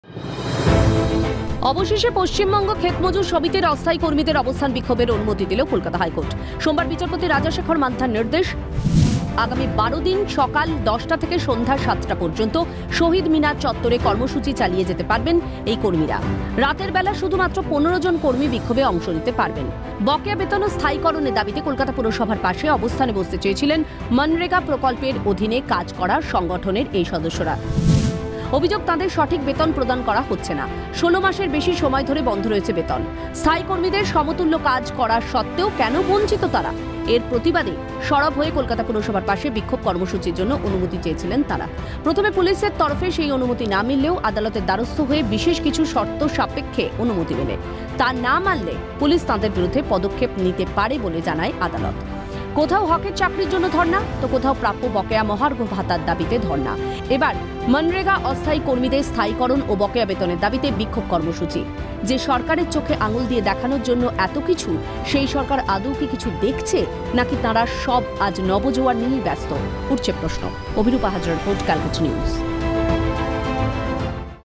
হাইকোর্টের অনুমতিতে বিক্ষোভ , অবস্থান বিক্ষোভে মনরেগার কর্মীরা | CN
পশ্চিমবঙ্গ ক্ষেতমজুর সমিতির অস্থায়ী কর্মীদের অবস্থান বিক্ষোভের অনুমতি দিল কলকাতা হাইকোর্ট। শহিদ মিনার চত্বরে শর্তসাপেক্ষে অবস্থান কর্মসূচির অনুমতি মিলল মনরেগার অস্থায়ী কর্মীদের।